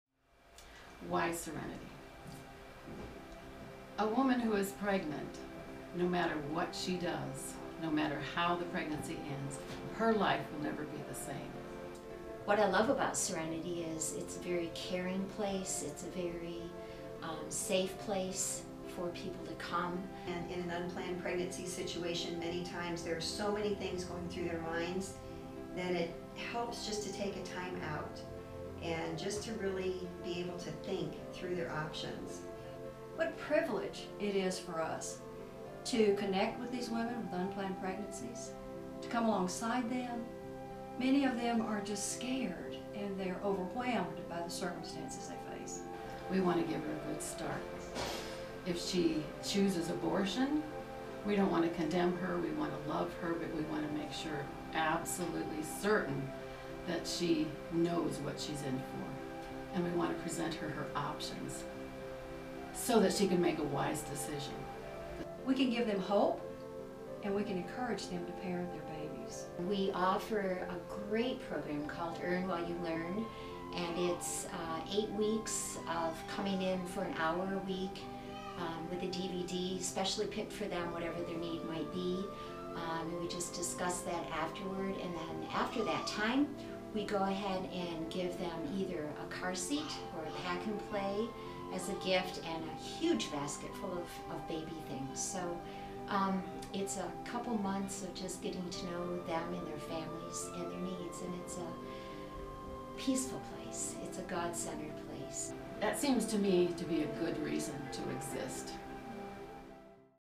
Serenity Pregnancy Resource Center Offering Hope to the Big Horn Basin
Serenity Pregnancy Resource Center is offers a peaceful and confidential place for unplanned pregnancy families to find hope during this difficult time.  We are here to define the options available to ensure that you are at peace with your decision and ensure that your are pregnant through pregnancy testing, ultrasound, and counseling.